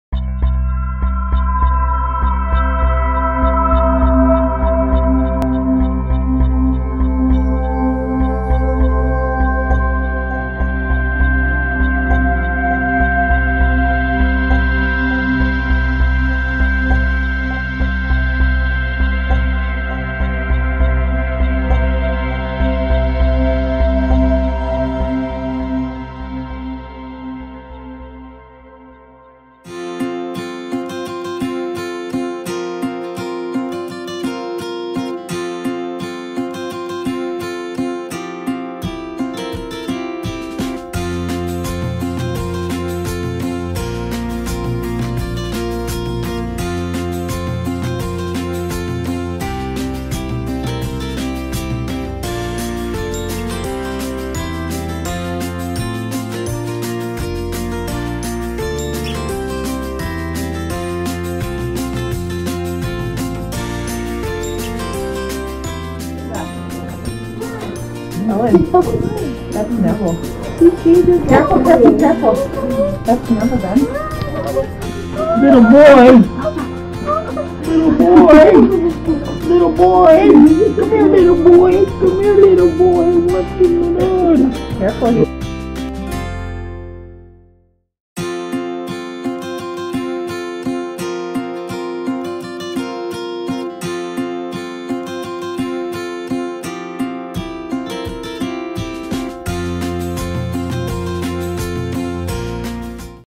Neville
Neville, a Yorkshire puppy, was saved from euthanasia due to our caring staff and donors.